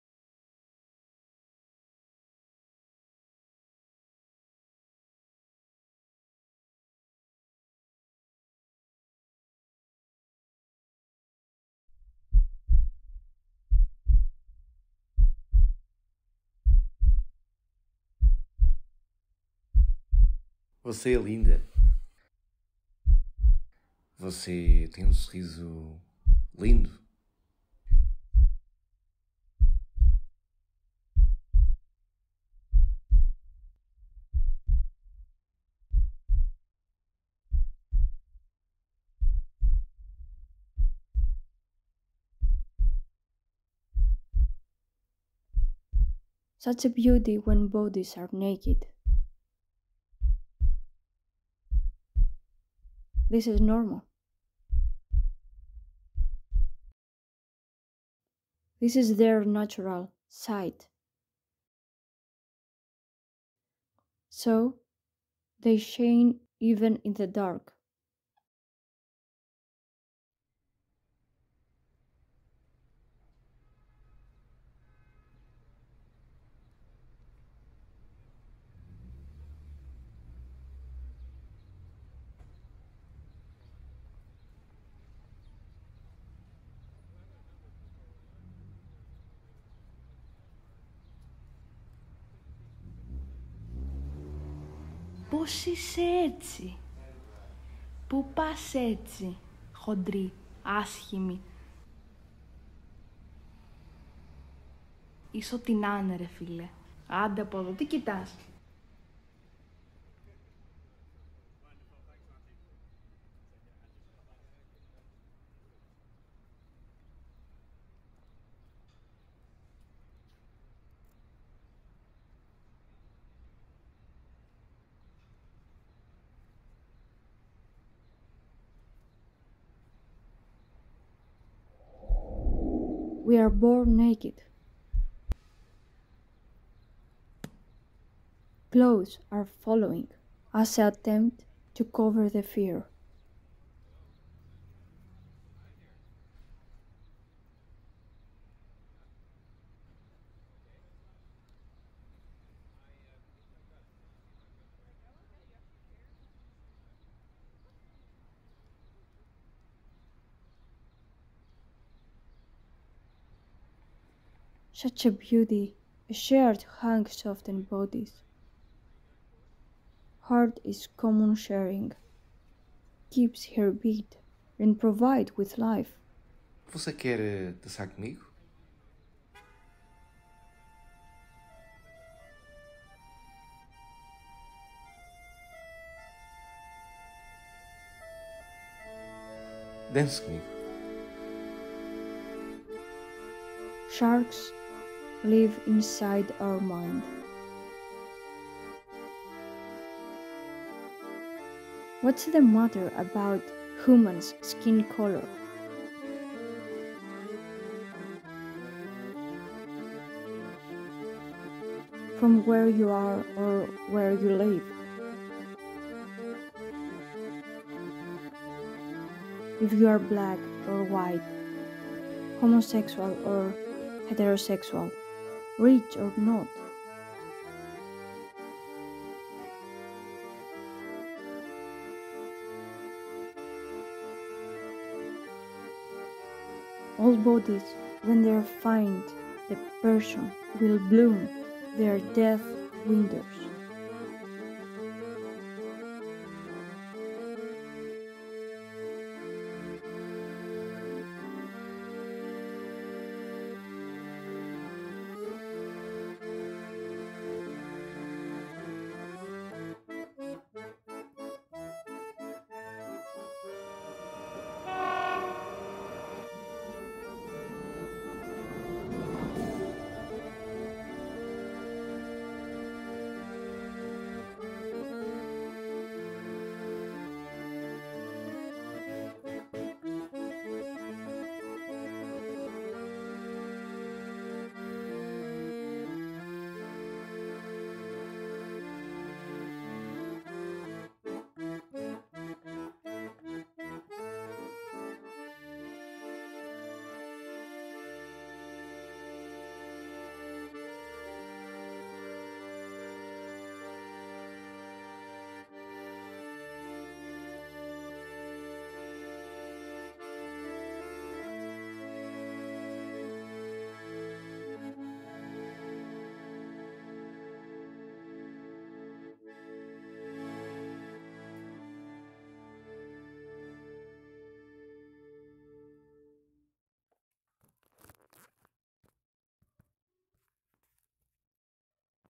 NAKED - a poem video by Evi Koroni (2022)
A short film project based on the poem NAKED.

Poem, Directed by Evi Koroni

Performer: Vassilis Ziakas, Maria Travlou, Sotiria Vokou, Evi Koroni

English poem narration by Evi Koroni
Portuguese narration by Nuno Franco
Greek voice of bullying by Maria Travlou

Filmed by Sotiria Vokou, Evi Koroni
Production: Très Romantique – art team
Co - production: Underground Pictures

Shot with the Canon EOS 2000D